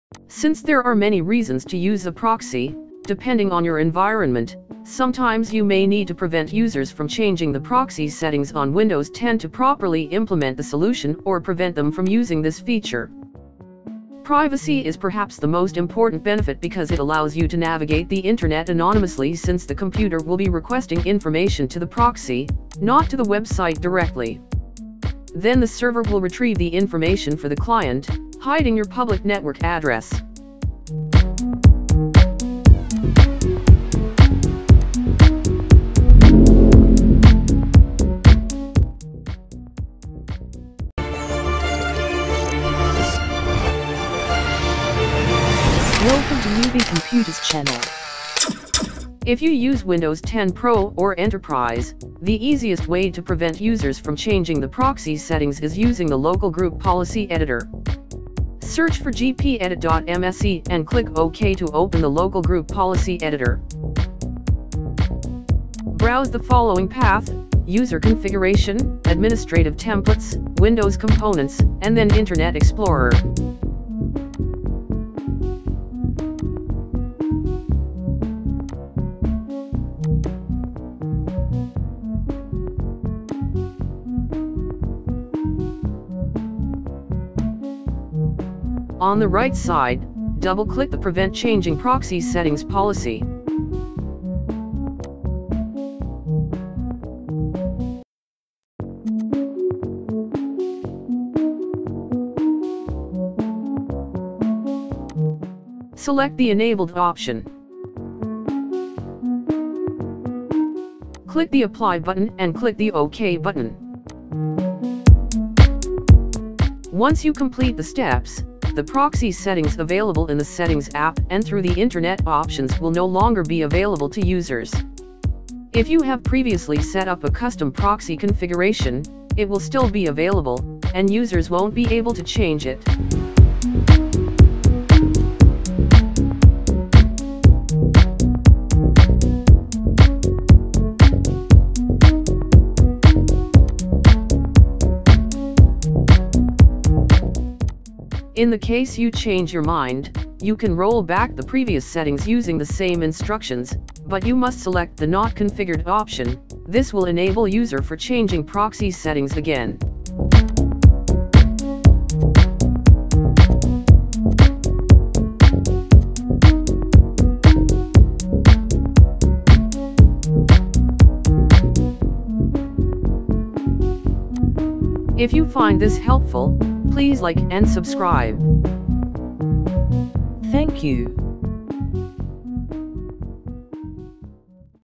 How to disable or enable proxy settings on Windows 10/windows 11 - group policy editor easy tutorial
Since there are many reasons to use a proxy, depending on your environment, sometimes you may need to prevent users from changing the proxy settings on Windows 10 to properly implement the solution or prevent them from using this feature.

00:48 let's go

block/restrict users from changing proxy